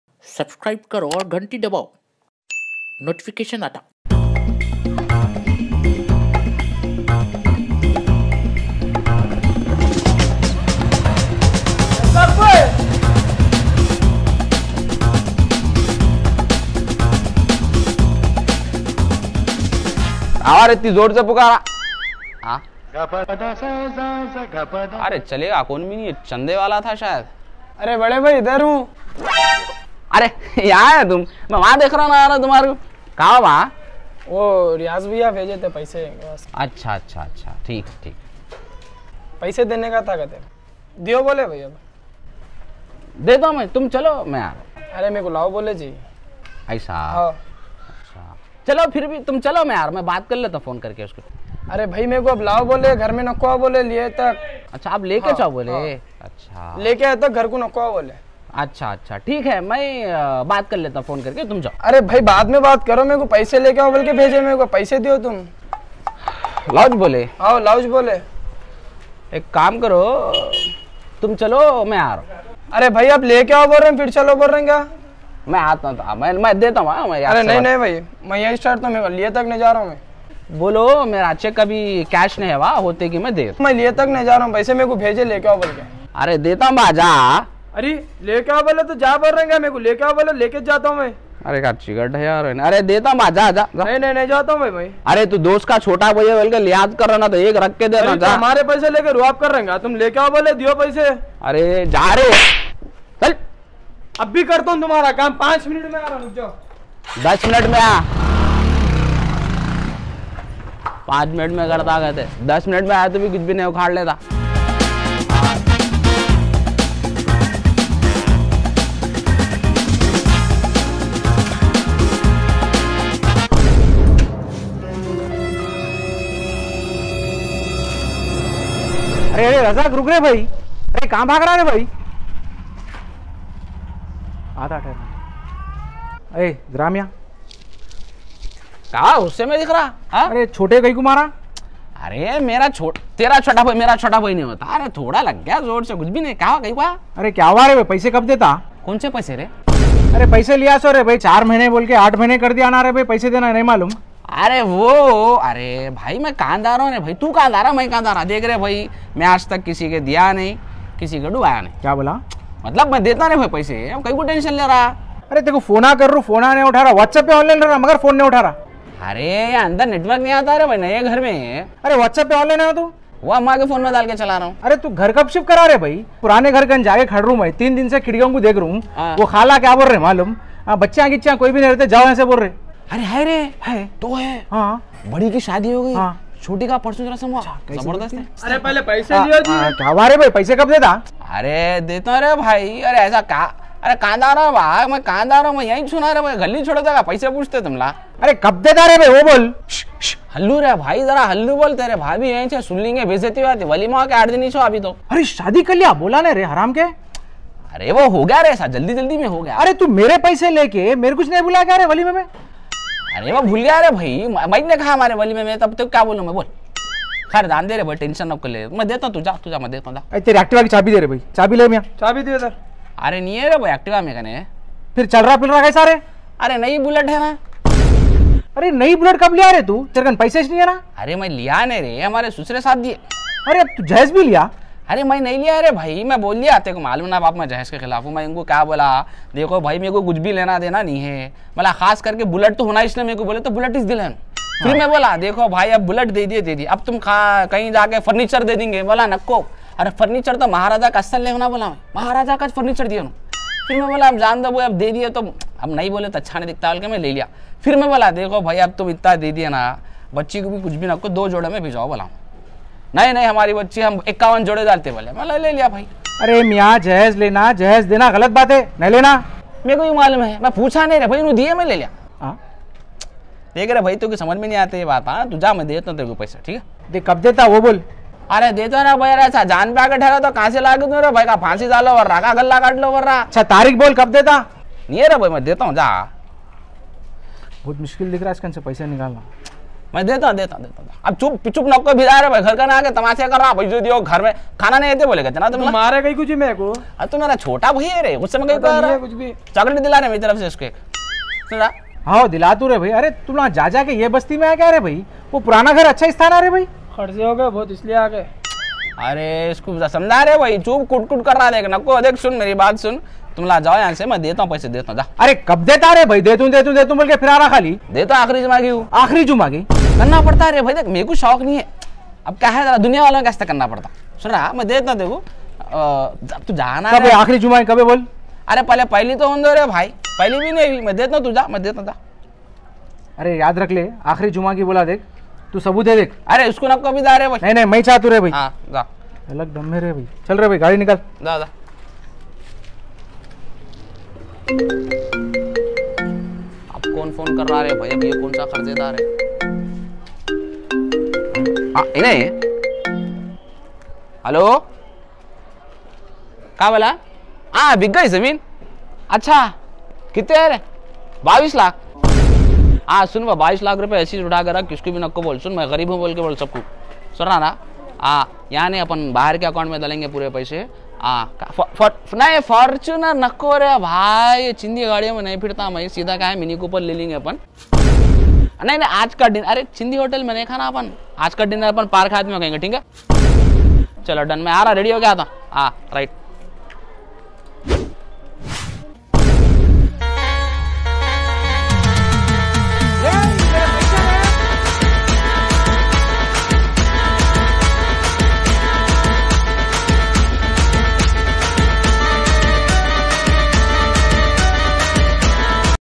Angry Kharze Daar | Funny Video 2021 | Hyderabadi Comedy Videos | Abdul Razzak | Golden Hyderabadiz
Angry Kharze Daar funny video 2021 ft. Abdul Razzak, Riaz Siddique aur Syed Shanoor. Say tuned to Golden Hyderabadiz channel for more yyderabadi comedy videos.

Cast :
Abdul Razzak
Riaz Siddique
Syed Shanoor
Concept by : Riaz siddique
Written and Edited by : Abdul Razzak